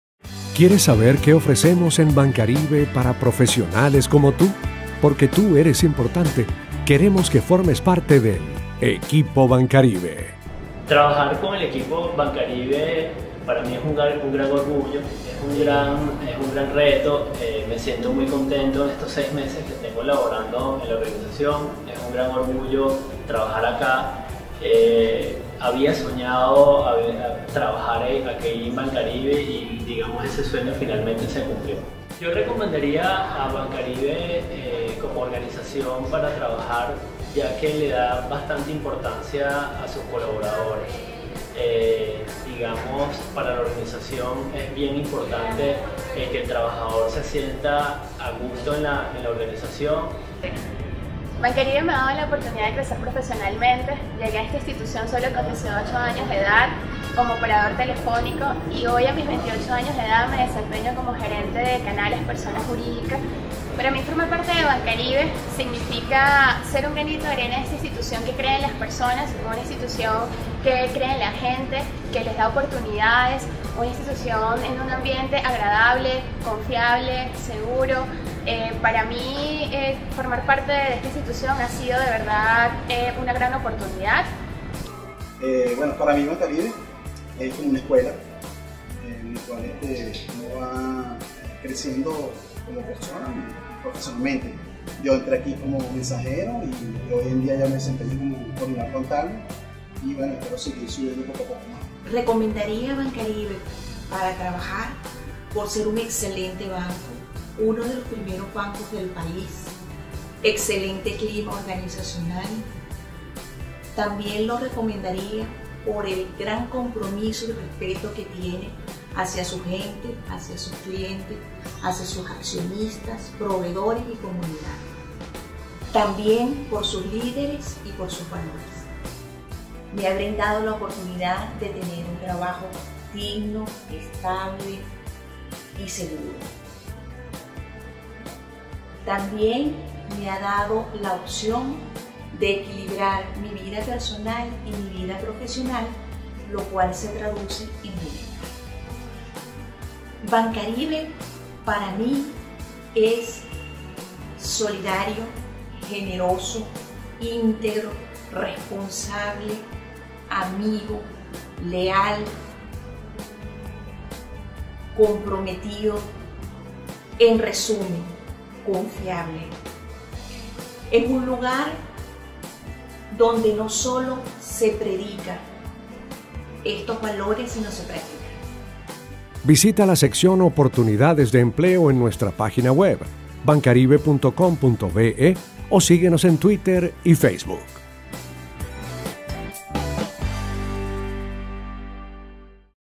Bancaribe: Progresamos contigo
Bancaribe promueve la creatividad y la innovación en sus trabajadores, además, impulsa que los trabajadores se sientan a gusto y puedan realizar una carrera fructífera y una larga trayectoria dentro de la institución. Aquí puedes conocer los testimonios de trabajadores del banco sobre su desarrollo laboral y profesional en la institución.